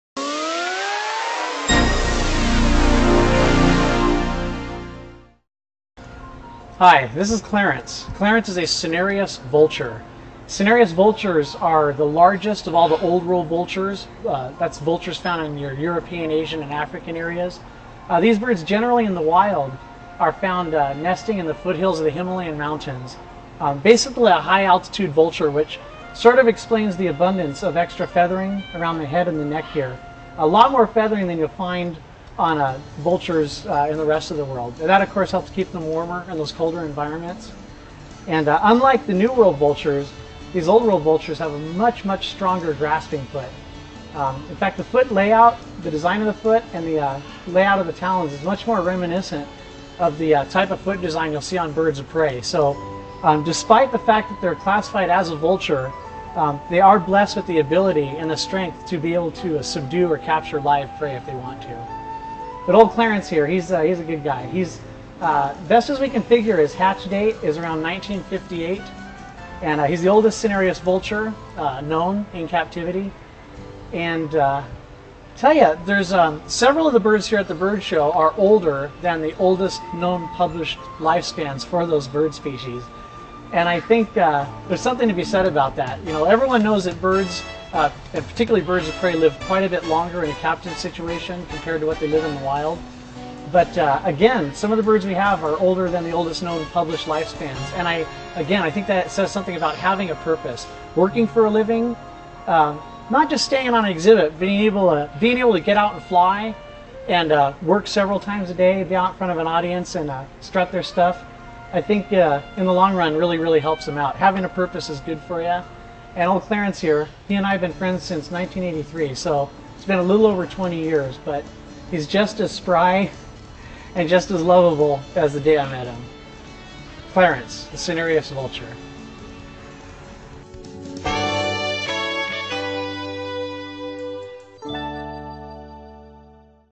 Cinereous Vulture
A short close up movie of Clarence the Cinereous Vulture with Kent Mahaffey. Berwick Productions.

Thank you for your support in caring for our birds.